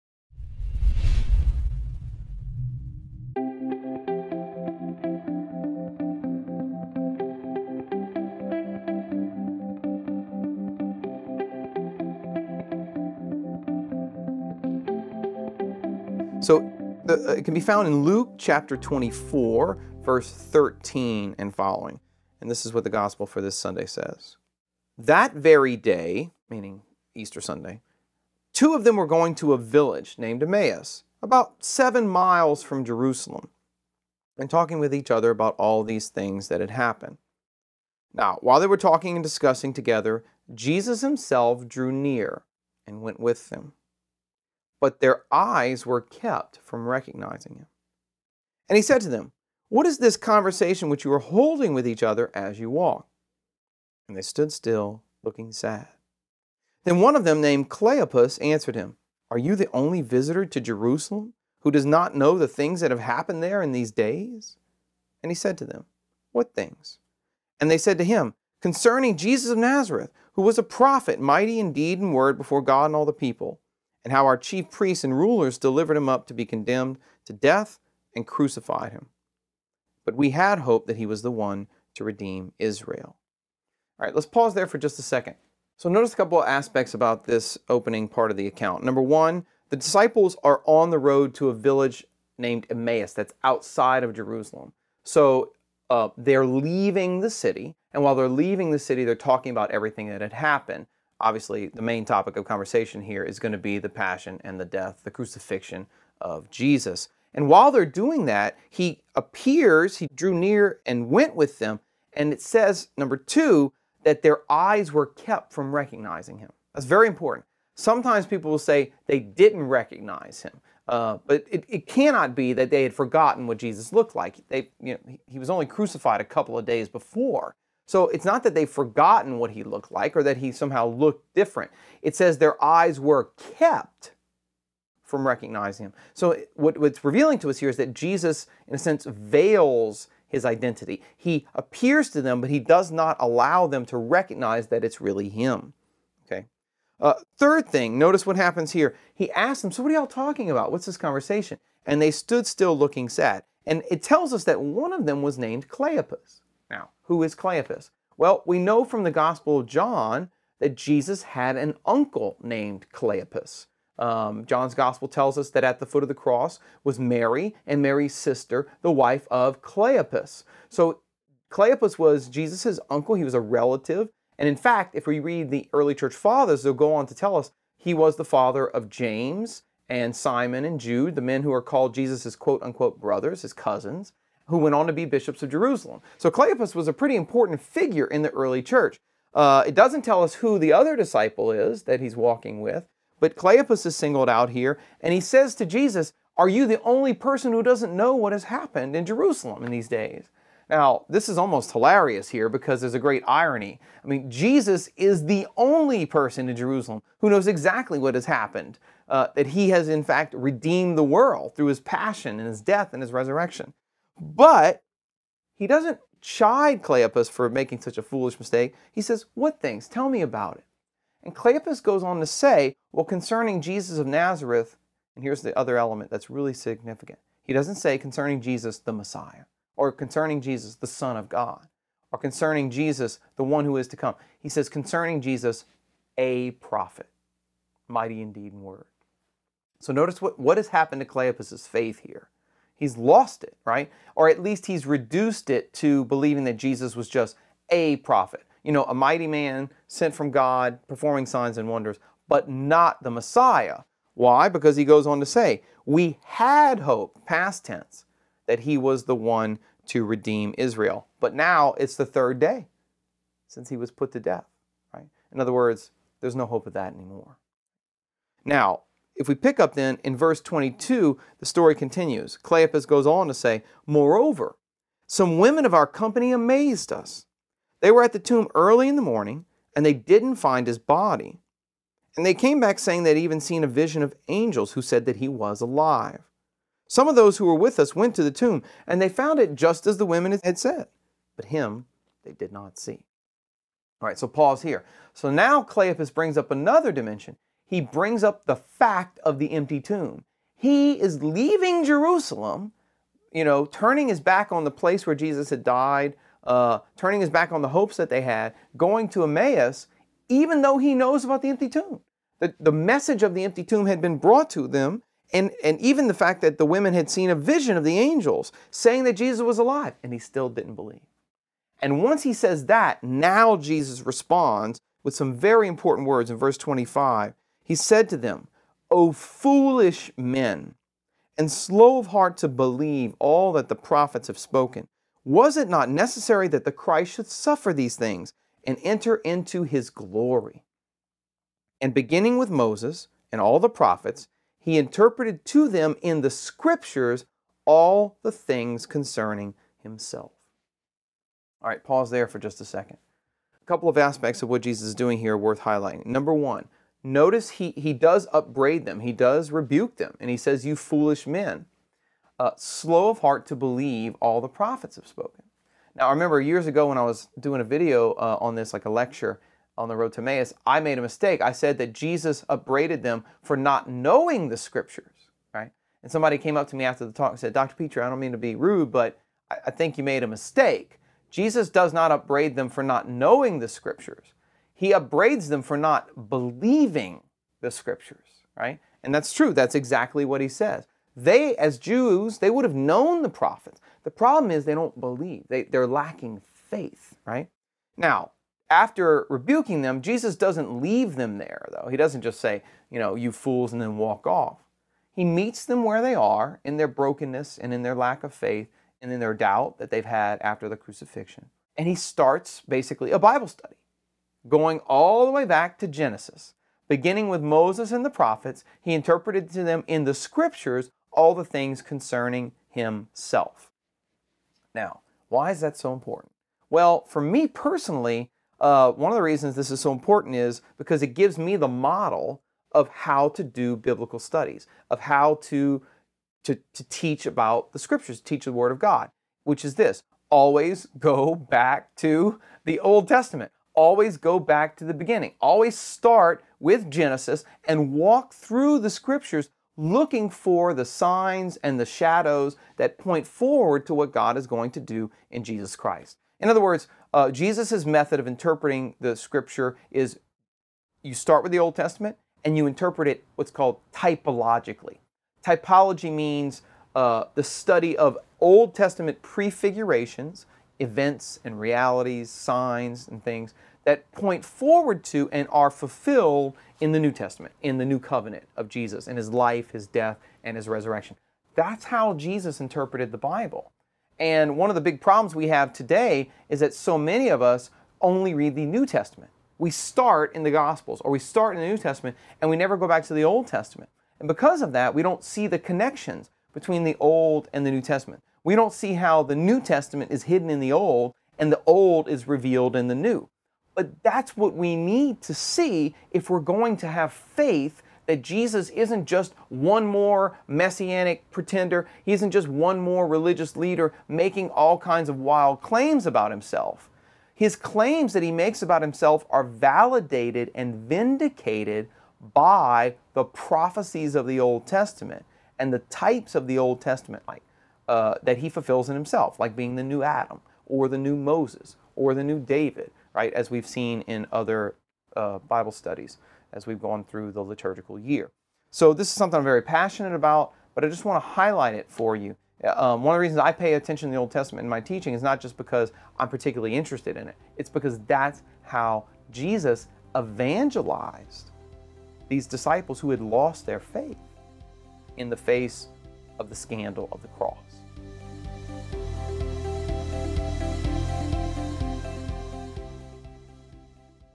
The Road to Emmaus
In the Road to Emmaus in Luke 24, Jesus approaches two disciples who seem to have lost faith in Jesus' promises.

Ironically, in the Road to Emmaus, these two disciples ask Jesus, when he inquires what they're talking about, if he hasn't heard the news about what happened to Jesus of Nazareth just a few days before -- and of course, he's the only one who really knows what happened.

On the Road to Emmaus, Jesus lays out a Biblical principle that should be employed by all who wish to understand and fell the full impact of the Gospels: going back to the Old Testament to discover how Jesus fulfilled the promises of the Old Testament.

Check out this video with Dr. Pitre on the Road to Emmaus to learn more.

To learn more about the Road to Emmaus in Luke 24, along with the connection between the Gospel reading, Responsorial Psalm, and the Old Testament reading for this Sunday's Mass, subscribe today to The Mass Readings Explained: